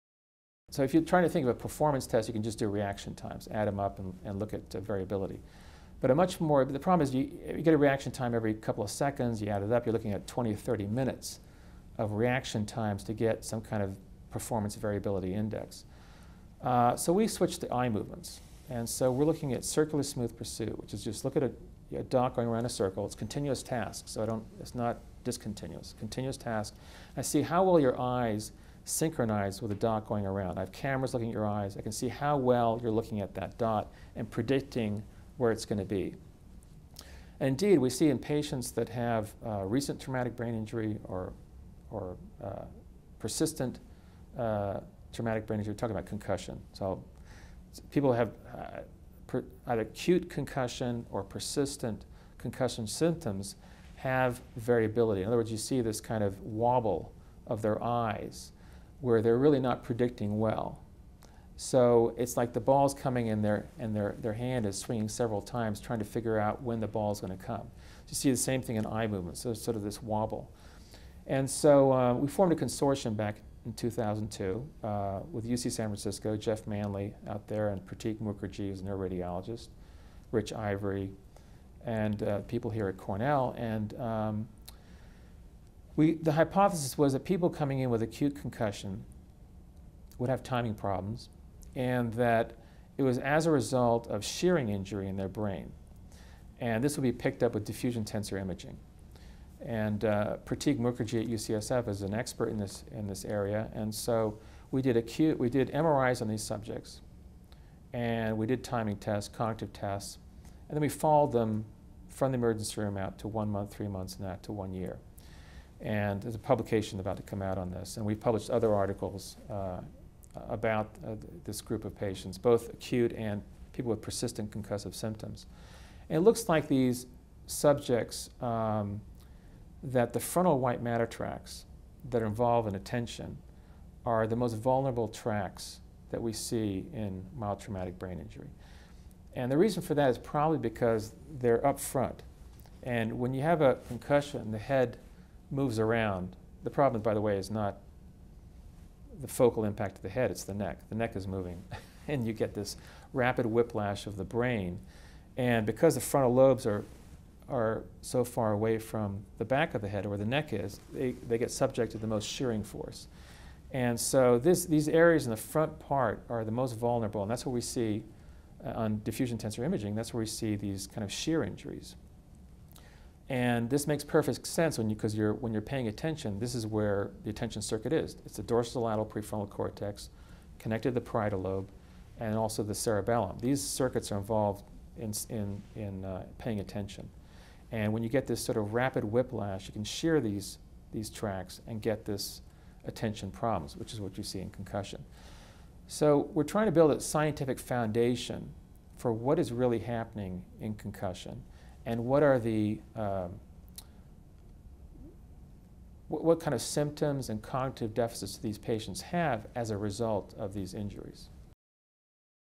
Dr. Jamshid Ghajar: Understanding the Nuances of Shearing Injuries in the Brain
Shearing is the stretching and tearing of the tiny nerve cells that comprise the brain. Learn more about the research and neuroimgaging that shearing can cause when the brain is injured.